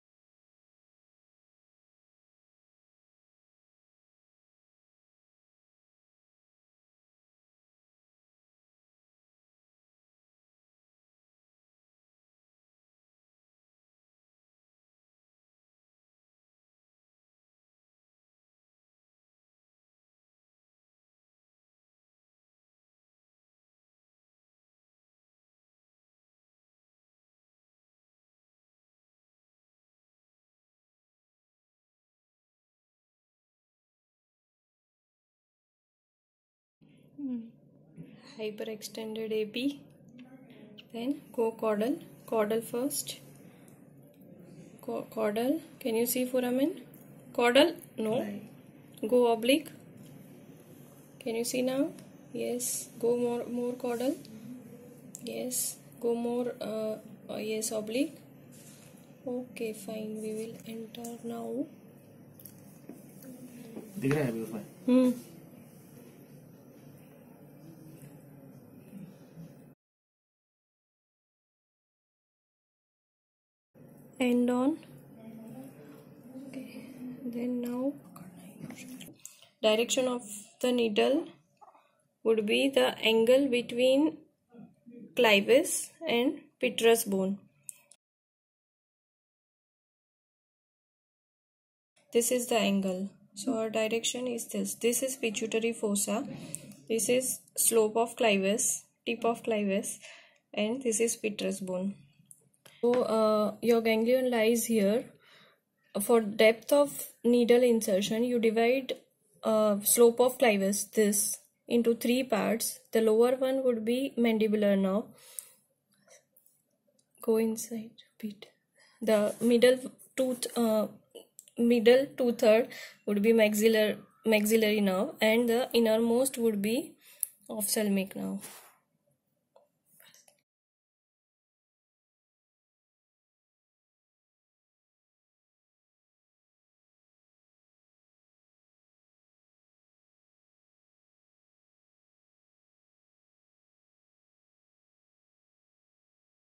Gasserian ganglion Trigeminal block - made simplified by demo on the skull
This video is demonstration of the anatomy, fluoroscope adjustment, bony anatomy, needle direction and depth. I hope it will help students.